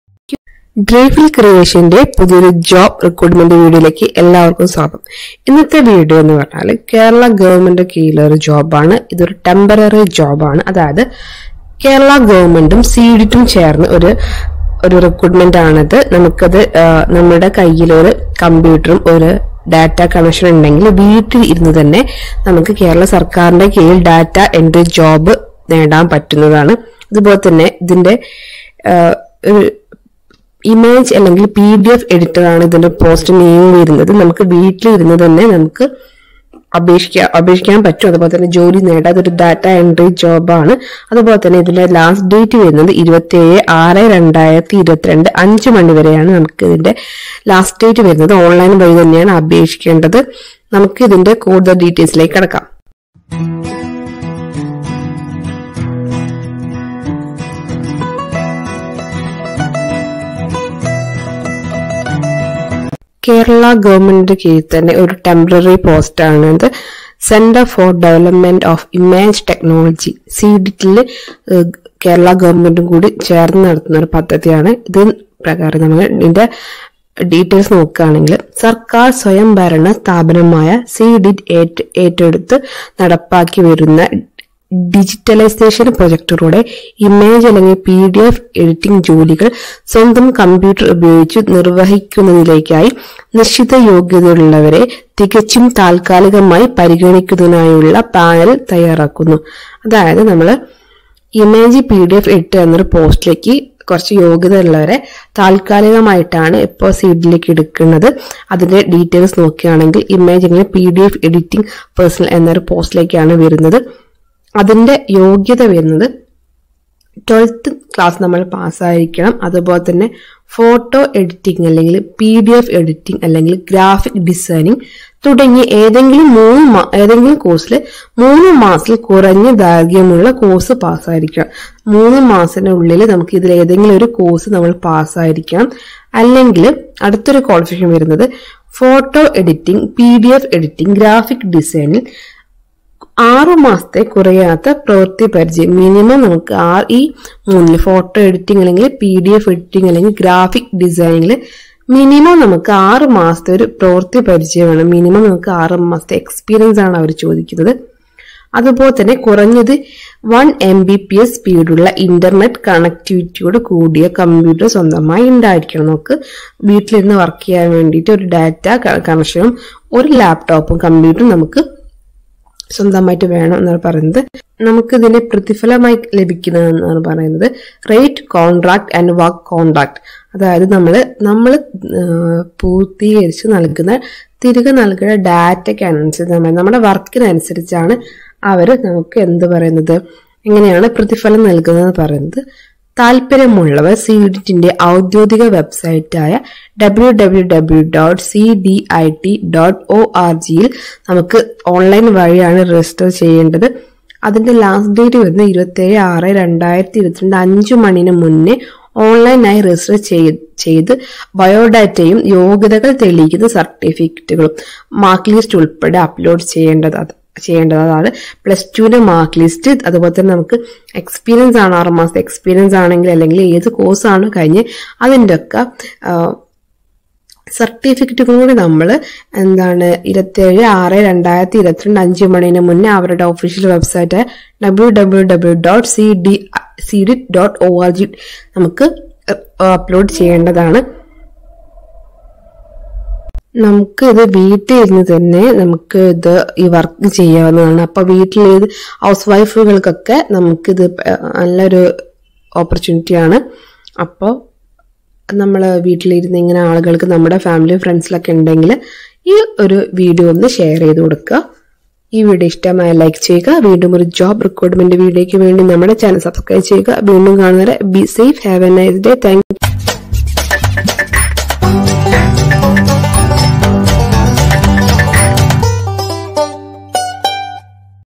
Data entry jobs|under government of Kerala|കേരള സർക്കാരിന് കീഴിൽ ഡാറ്റ എൻട്രി ജോബ് നേടാം| C-DIT
C - DIT invites applications from eligible candidates for empanelment as Image / PDF editing personnel